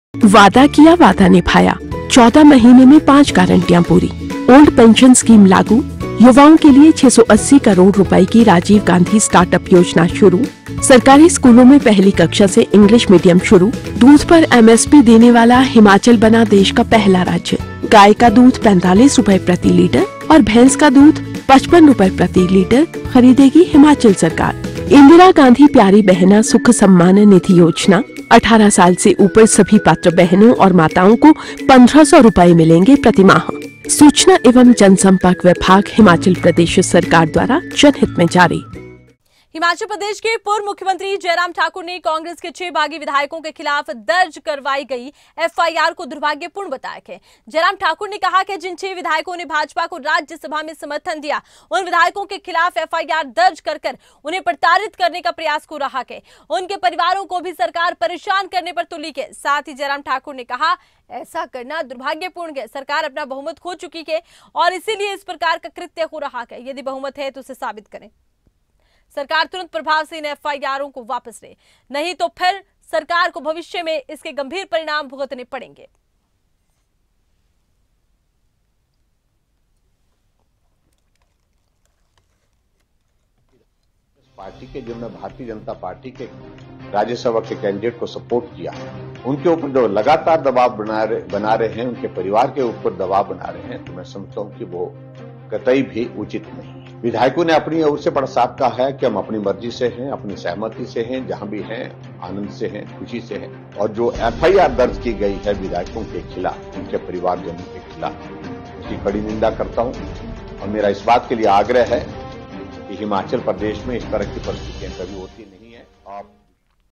Himachal: नेता विपक्ष जयराम ठाकुर ने कांग्रेस पर कसा तंज, विधायकों के खिलाफ केस करना दुर्भाग्यपूर्ण
Himachal: नेता विपक्ष जयराम ठाकुर ने कांग्रेस पर कसा तंज
विधायकों के खिलाफ केस करना दुर्भाग्यपूर्ण, सरकार वापिस ले FIR